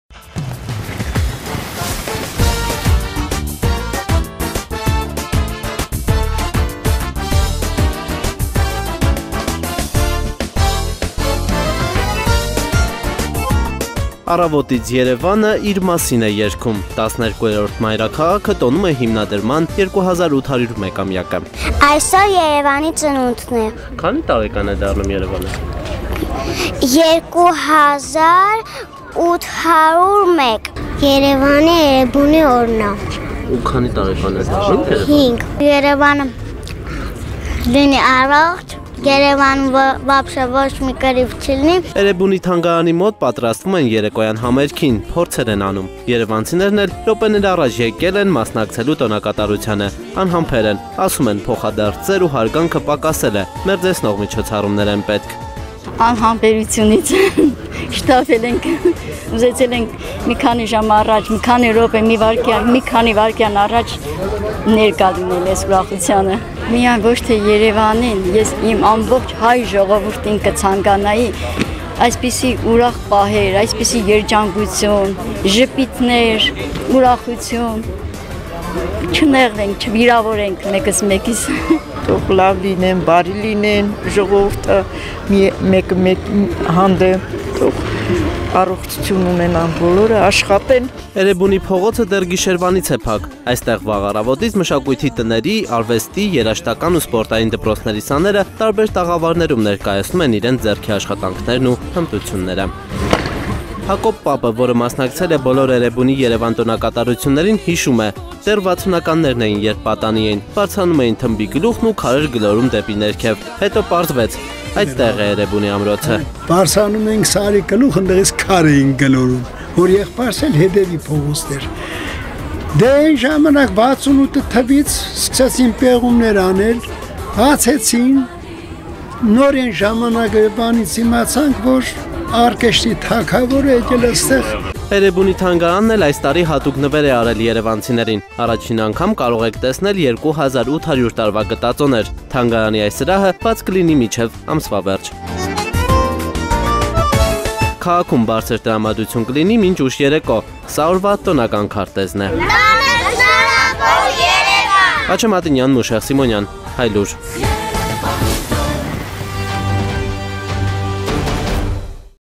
ԷՐԵԲՈՒՆԻՆ ԲԱՑԵԼ Է ԳԱՆՁԱՐԱՆԸ. ԵՐԵՎԱՆԻ ԾՆՈՒՆԴՆ Է
Այսօր մայրաքաղաք Երևանին սիրո խոստովանություն անելու օրն է: Էրեբունի-Երևանը նոր հարյուրամյակ է մտել 2801 տարեկան է: Մայրաքաղաքն այսօր կլսի 12 ոճի երաժշտություն ըստ վարչական շրջանների:
Երևանի փողոցներով երևանցիների տրամադրություններն է հավաքել Հայլուրի տեսախցիկը